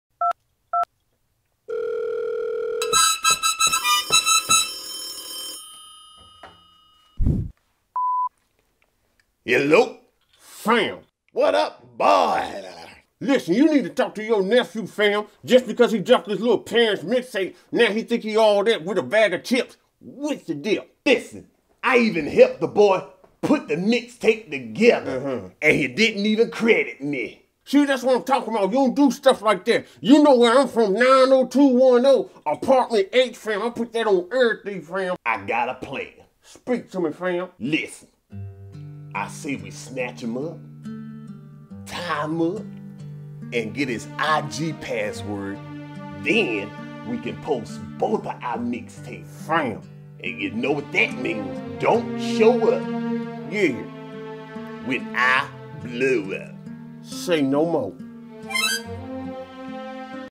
The Set Up (comedy skits) | Wright Films
Uncle Willy and Lil Boss are planning to set up Mello in this comedy skit